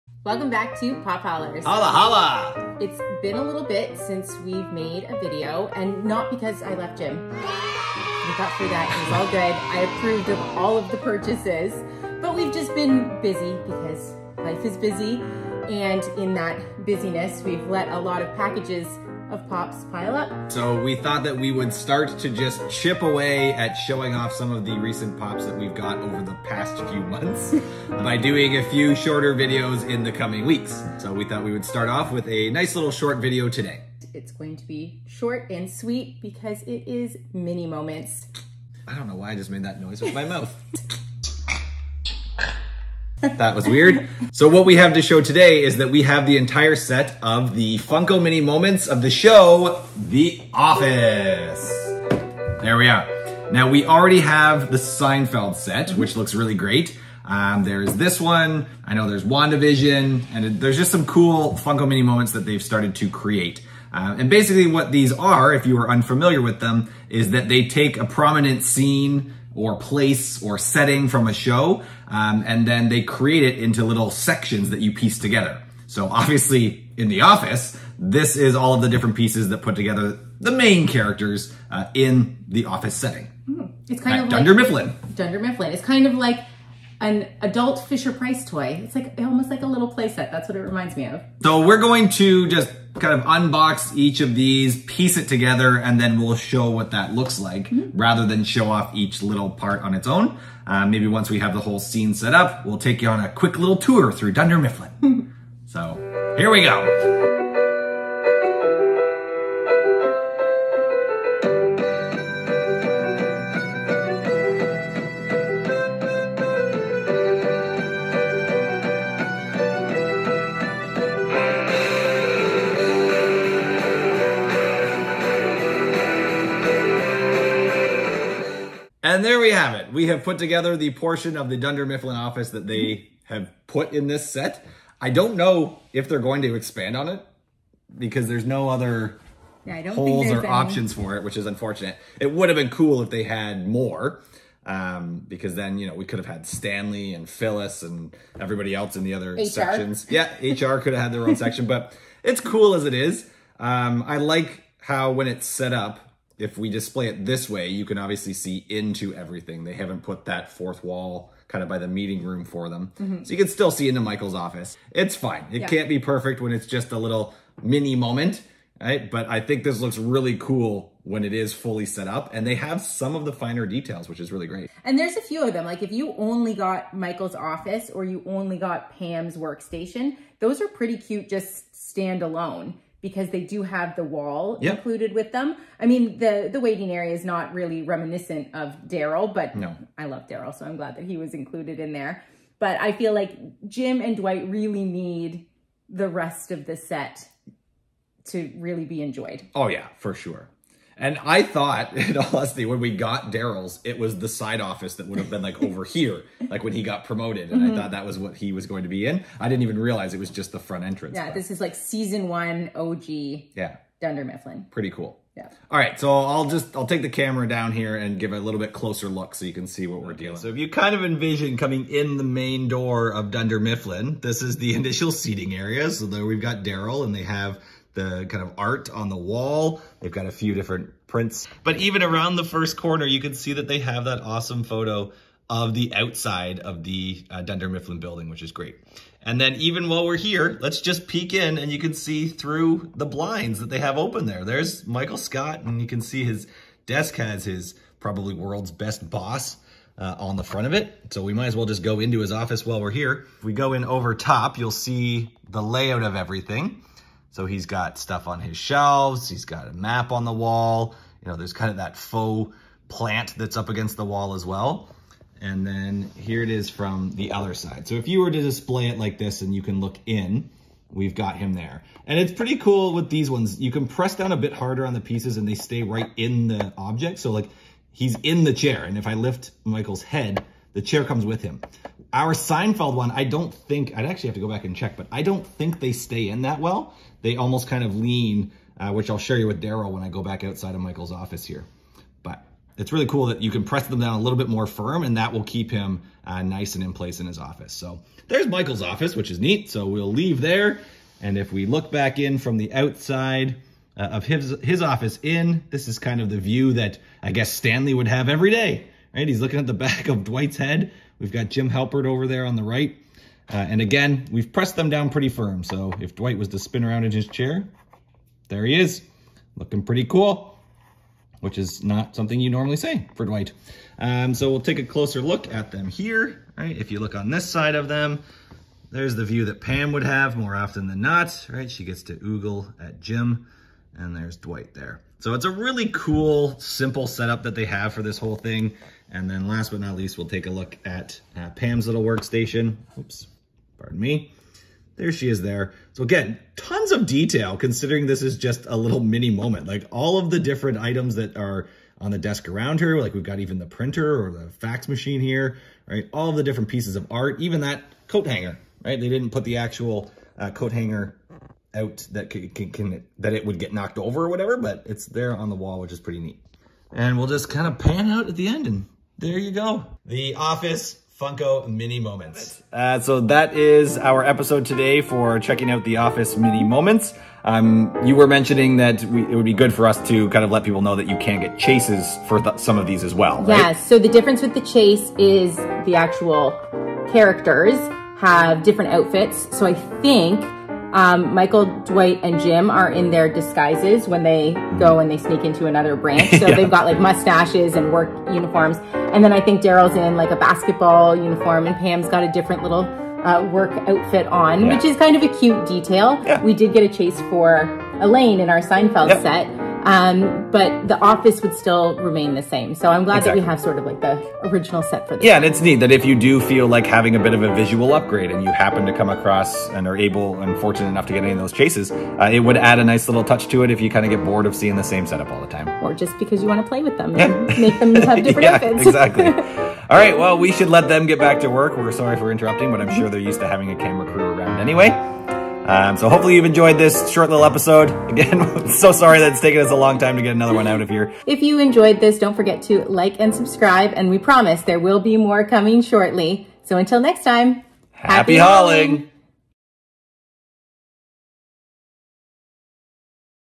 Take a Tour of Dunder Mifflin!
We are back with a short tour through some of the Dunder Mifflin office, thanks to this awesome Funko Mini Moment!